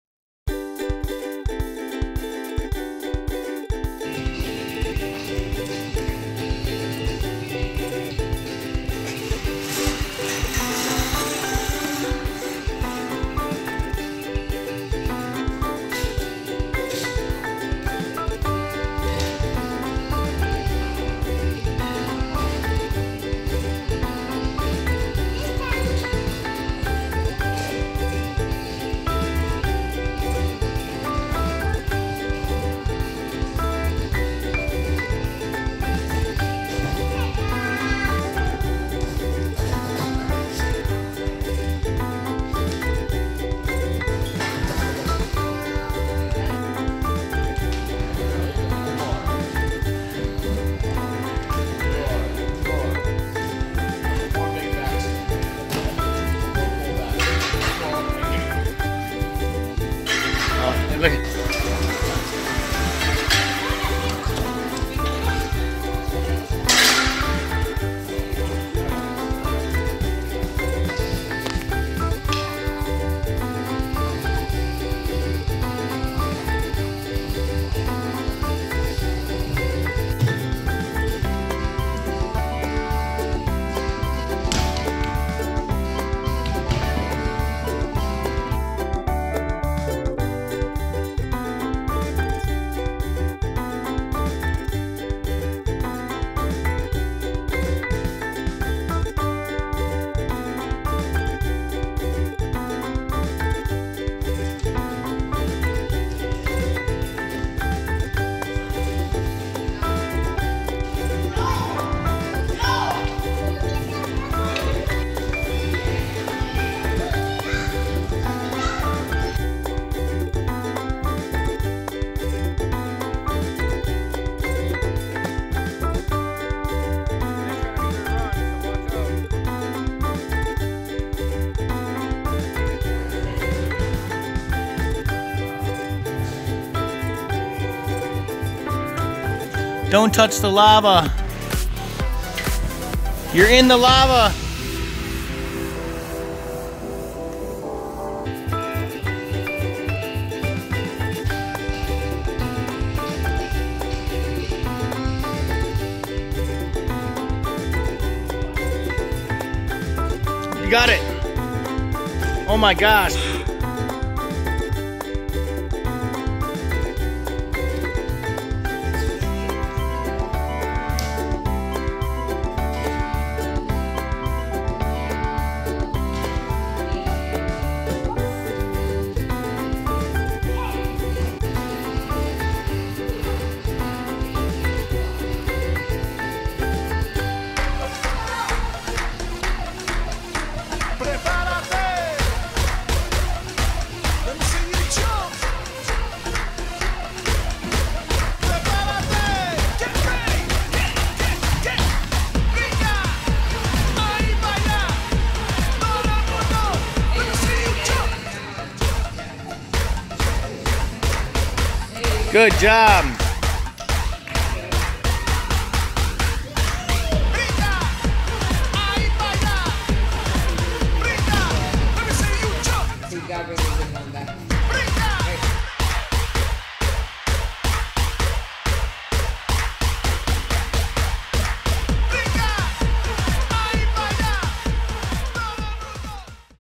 Ninja Warrior Kid Hits Obstacle Course For Training
Kid Ninja Warrior Obstacle course training. Addie Mae trains for American Ninja Warrior kids at Conquer Ninja course. She likes the warped wall and the rings.

☆Addie Mae Plays☆